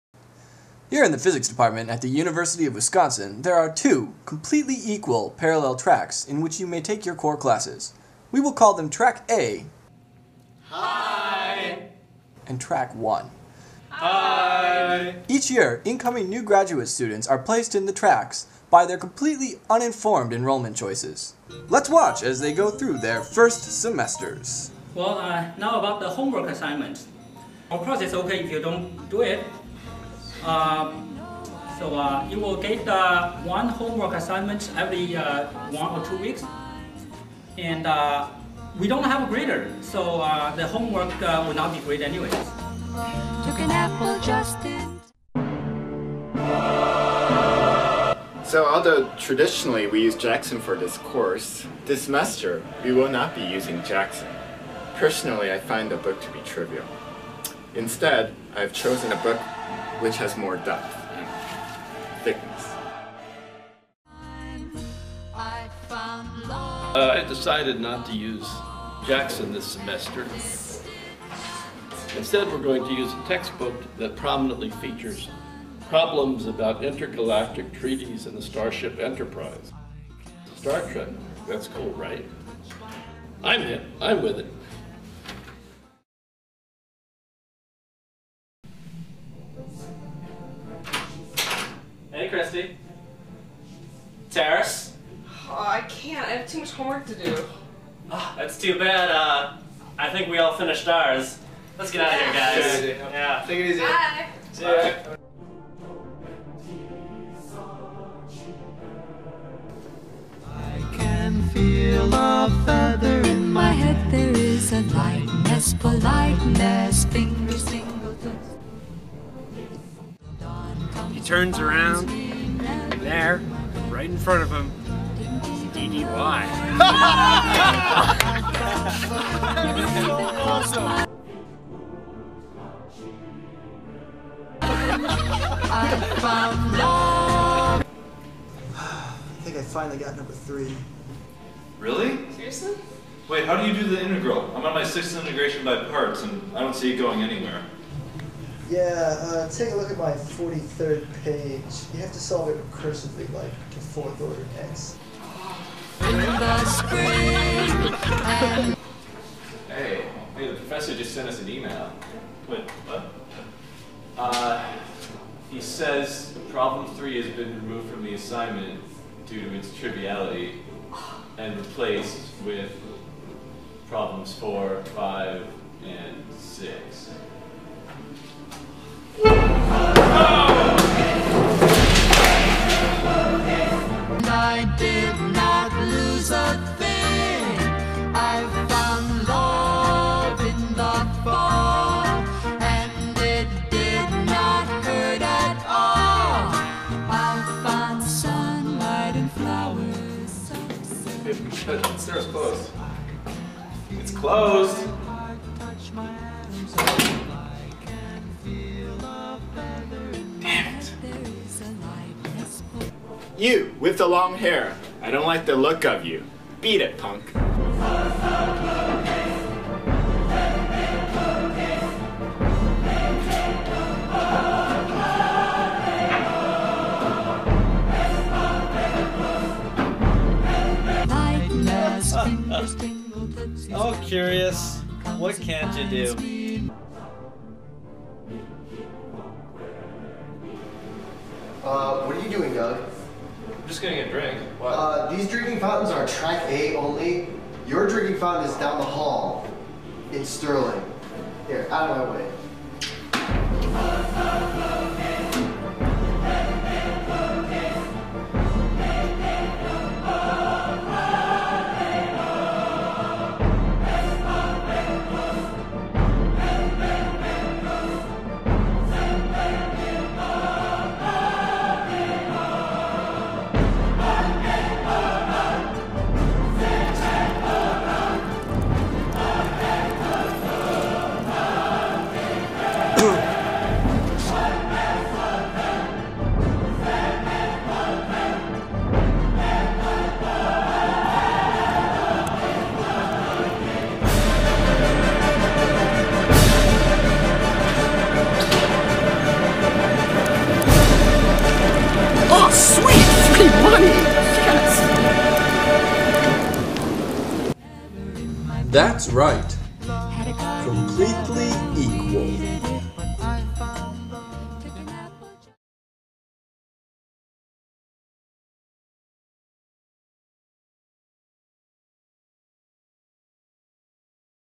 Two Tracks - Holiday Colloquium 2007
From the UW Physics Department Holiday Colloquium 2007

Two sets of students begin their journey through graduate school